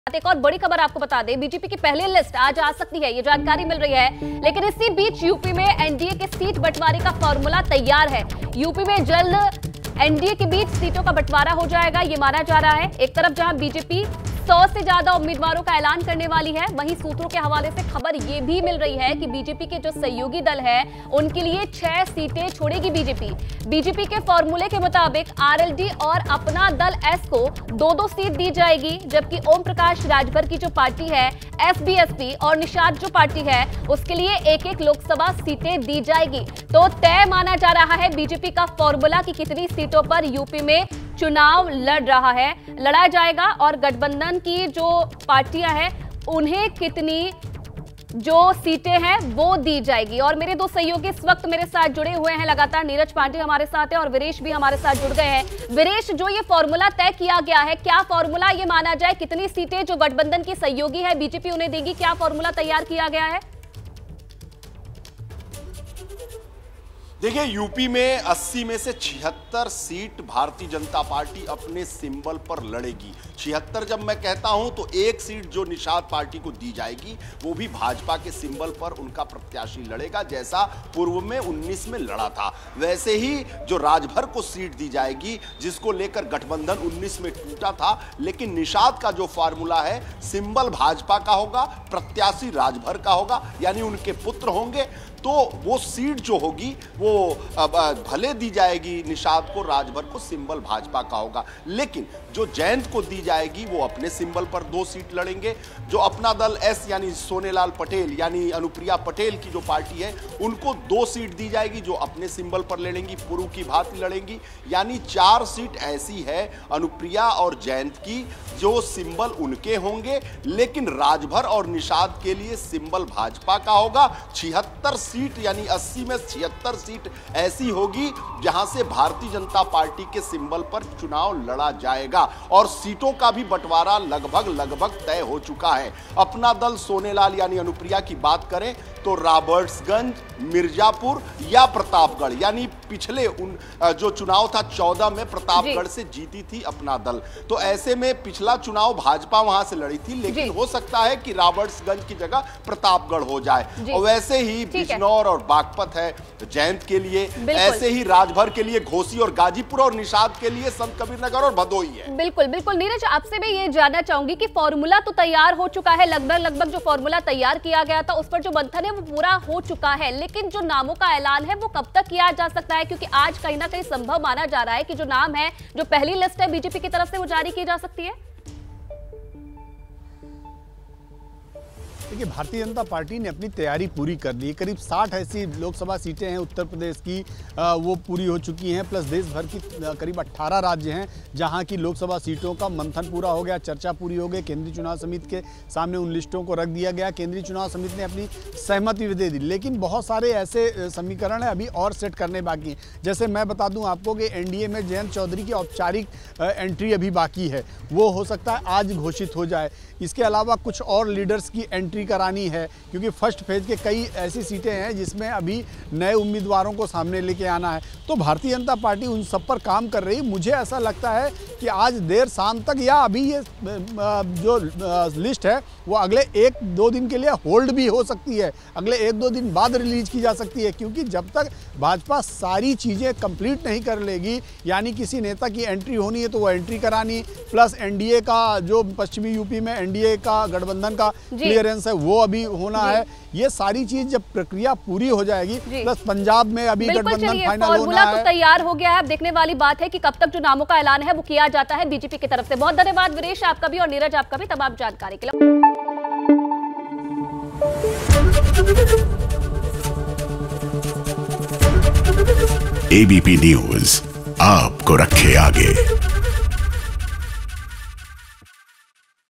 NDA Seat Sharing: UP में जल्द होगा NDA का सीट बंटवारा, सहयोगी दलों के लिए बीजेपी छोड़ेगी इतनी सीटें
लोकसभा चुनाव की तारीखों के ऐलान से पहले सभी पार्टियां अपने अपने उम्मीदवारों की लिस्ट जारी कर रही है..इस बीच अब खबरें है की एनडीए भी यूपी में जल्द ही सींट बंटावरा करेगी..माना जा रहा है की बीजेपी खुद 100 उम्मीदवारों को उतारेगी और अपनी सहयोगी पार्टियों के लिए 6 सीटें खाली रखेगी.